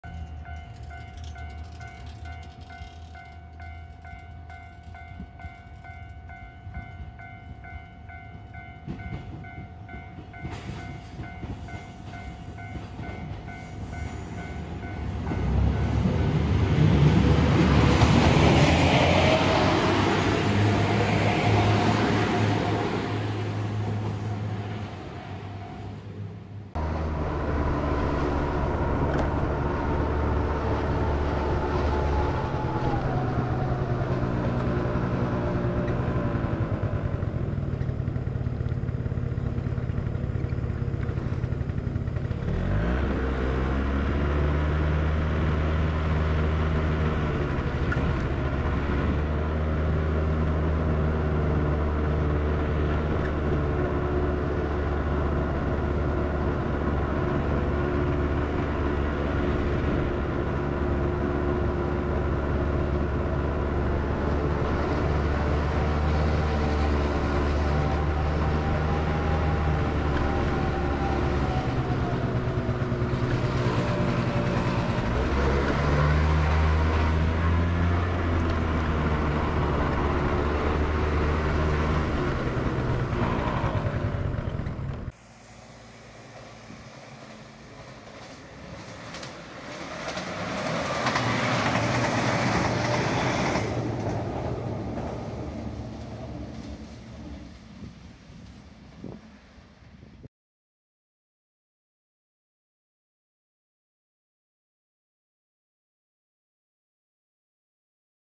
2700系 旭駅発車と中須賀土地区画整理
0:00旭駅
0:20北川踏切
0:37中須賀土地区画整理
1:27久万川橋梁